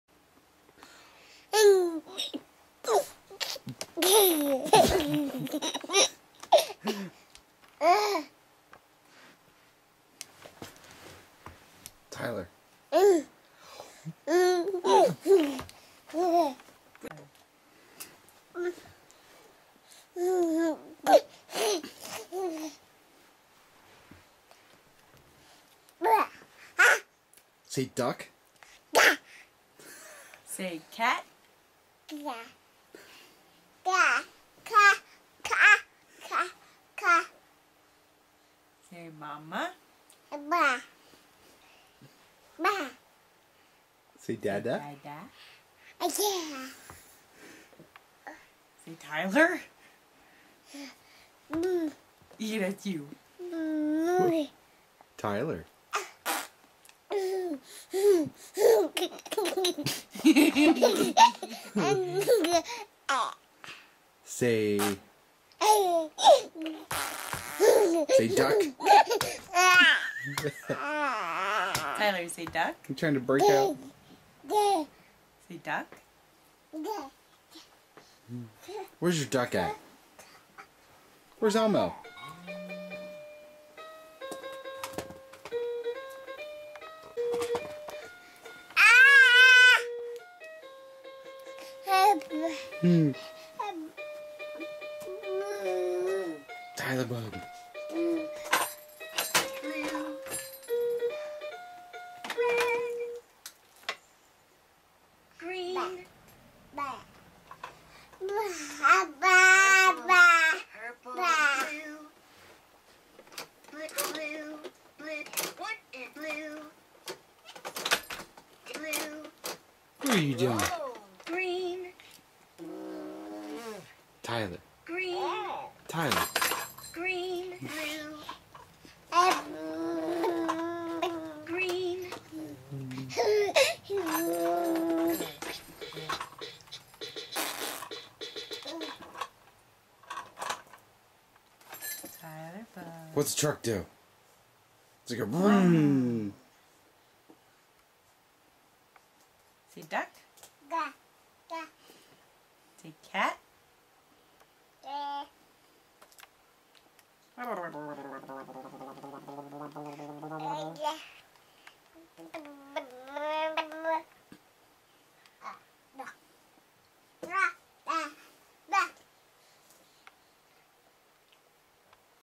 Tyler talking
Tyler trying to say duck, cat, etc